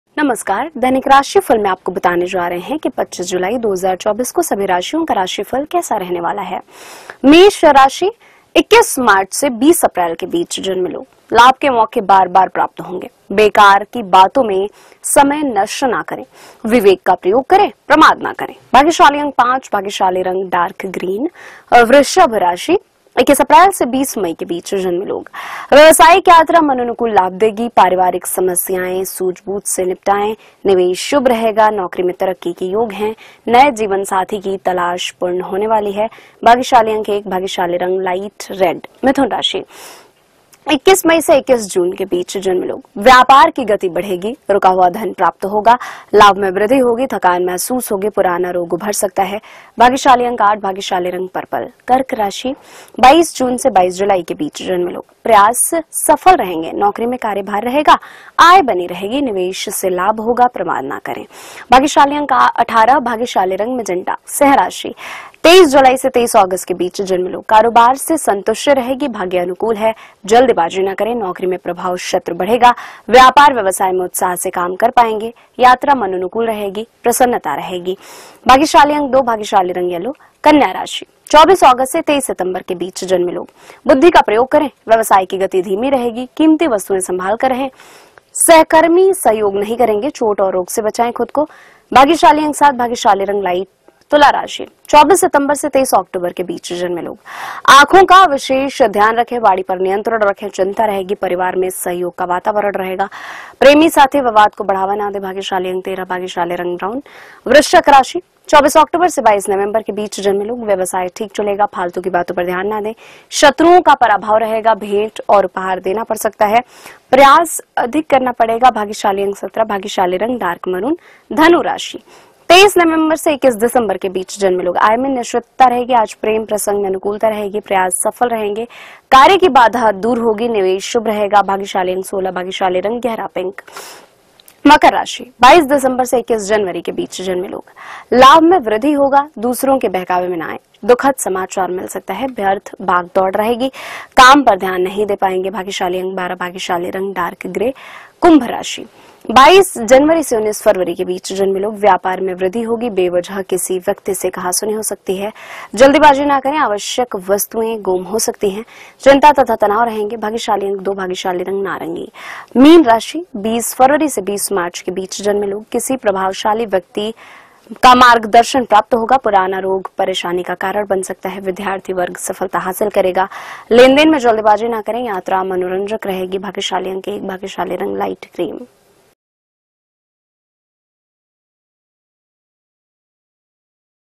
Aaj Ka Rashifal 25 July: आज का दिन इन राशि वालों के लिए है खास । Astrology । 12 राशियों का राशिफल
दैनिक राशिफल में आपको बताने जा रहे हैं कि 25 जुलाई 2024 को सभी राशियों का कैसा है राशिफल?